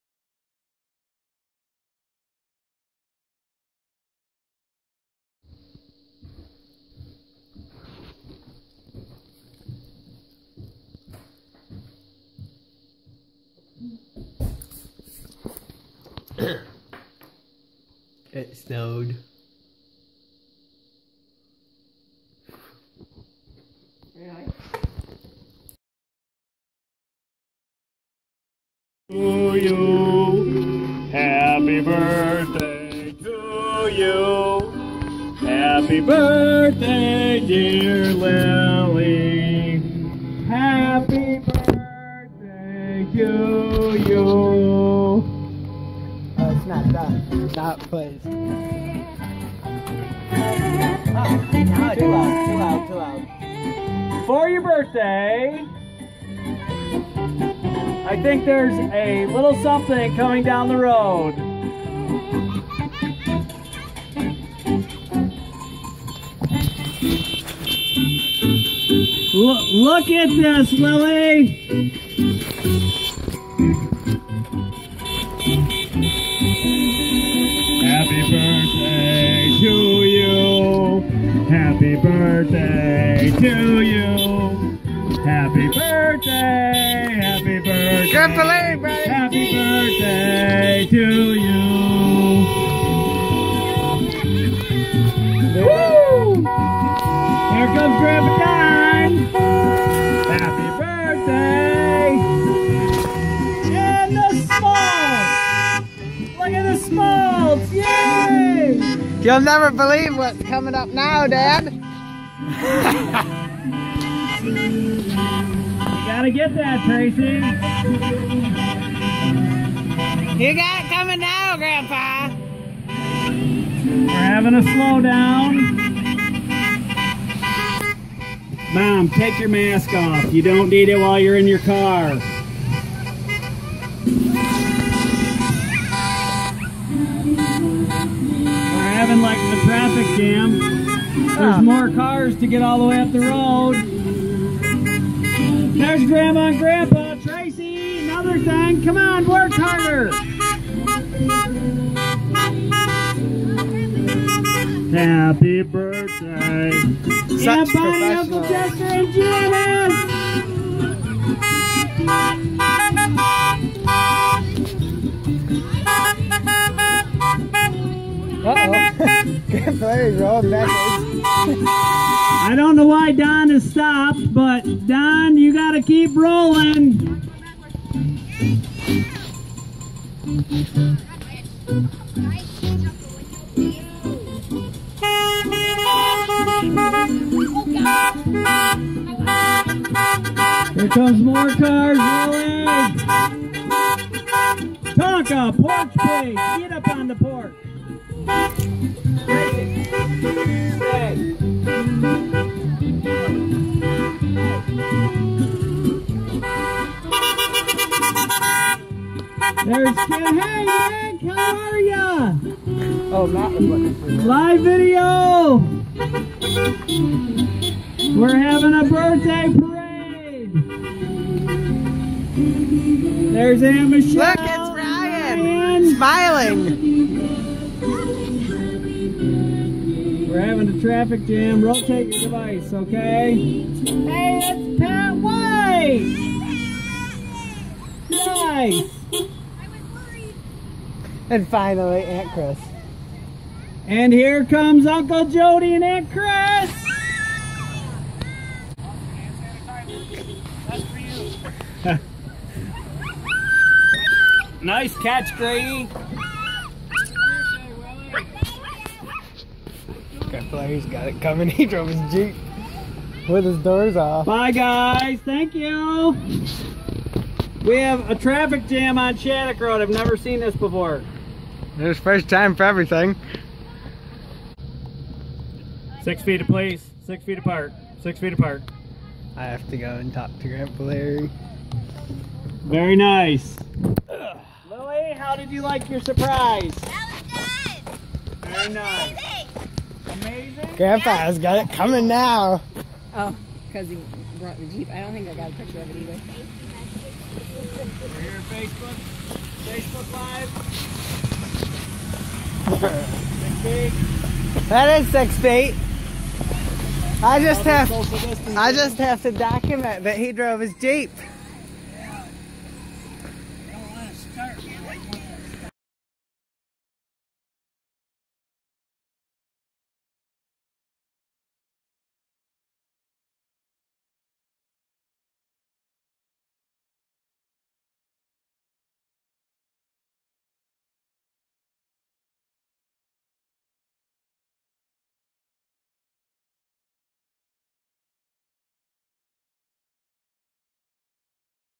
lilys birthday suprise
it was my sisters birthday and because of quarantine she couldn't see her friends.  so we did this.